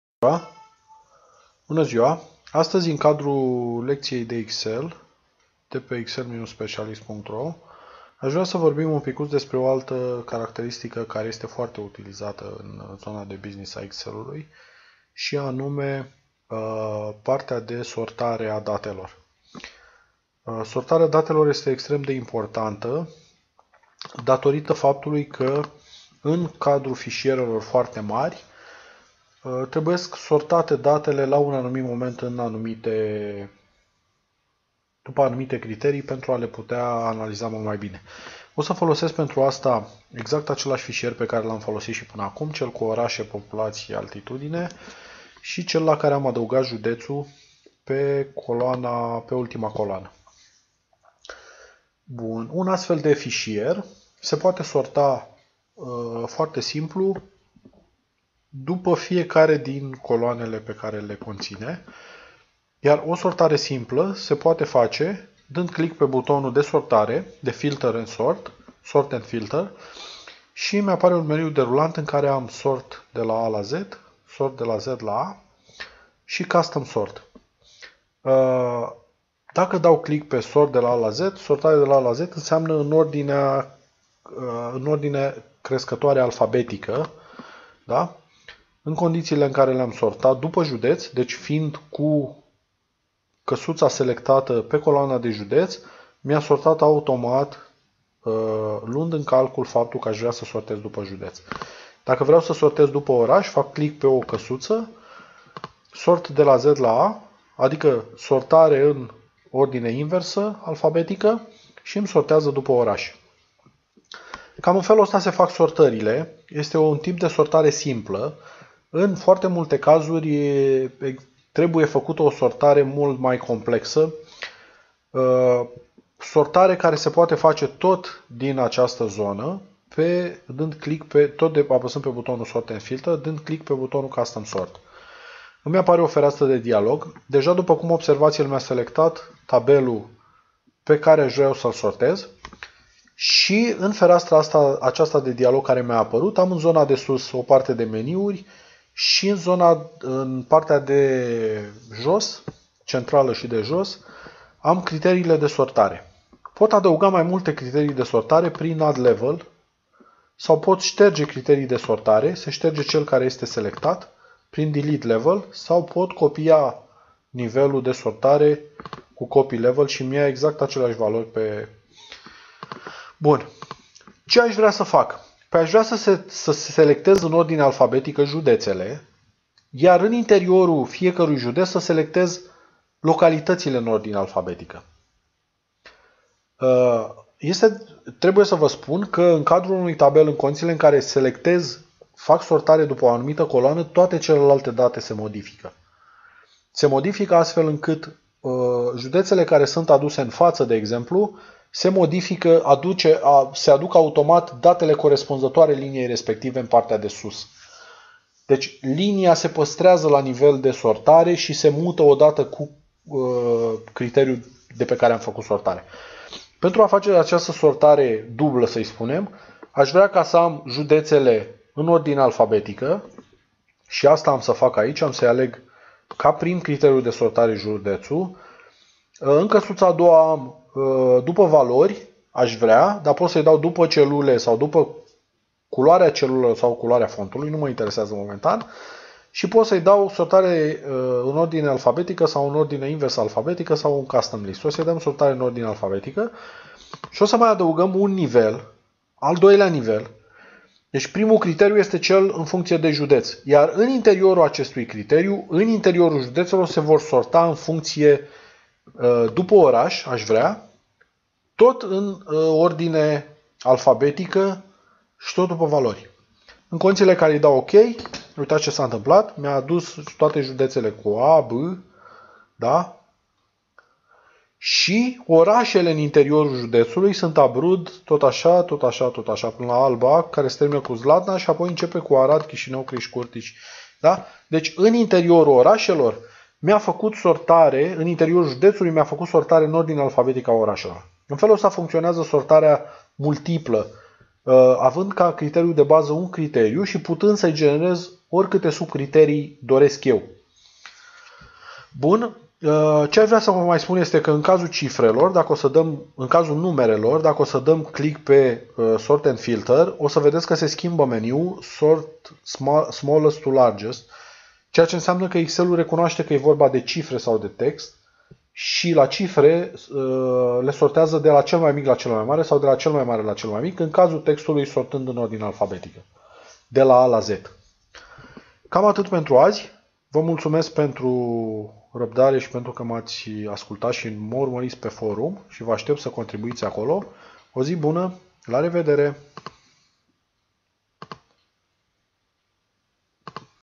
sortari in excel
sortari in excel, sortari simple si sortari pe criterii multiple.